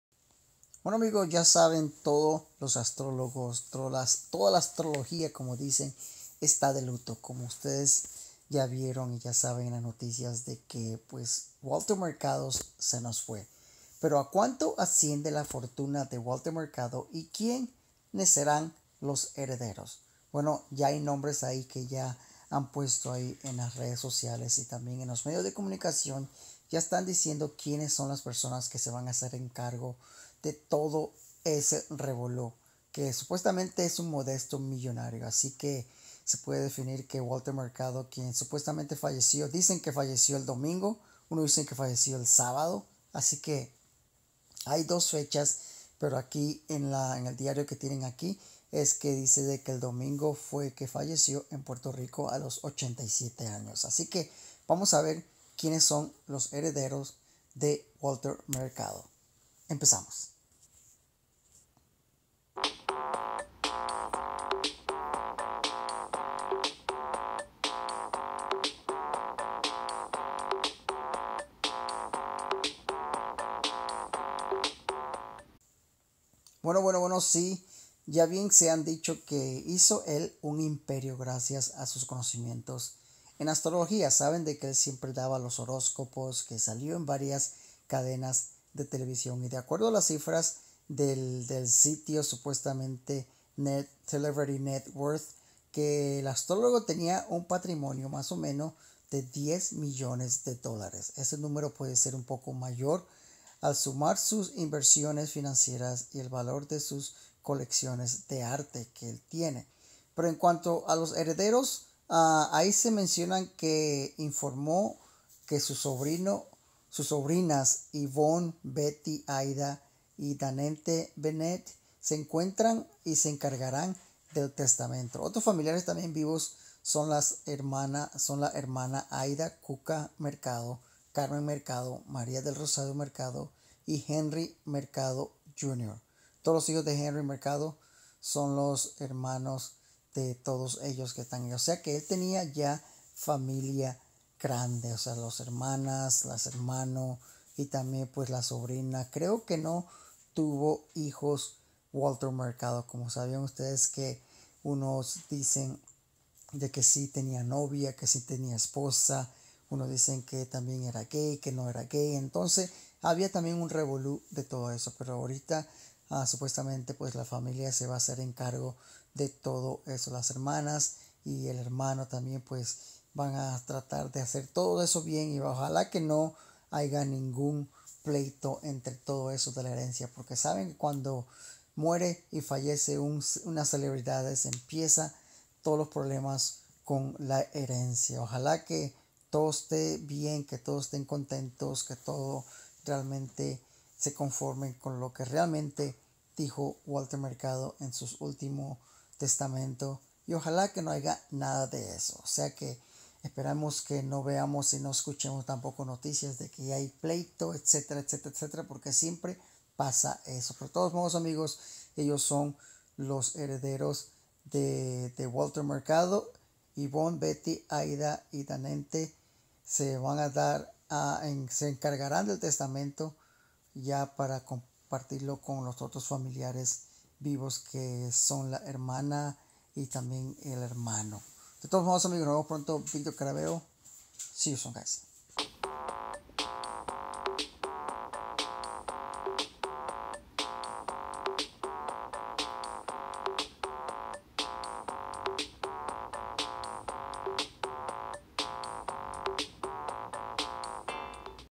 ¿A cuánto asciende la fortuna de Walter Mercado y quién la heredará?
¿A cuánto asciende la fortuna de Walter Mercado y quién la heredará? Walter el astrólogo Walter Mercado, quien falleció el domingo en Puerto Rico a los 87 años. falleció el domingo en Puerto Rico a los 87 años.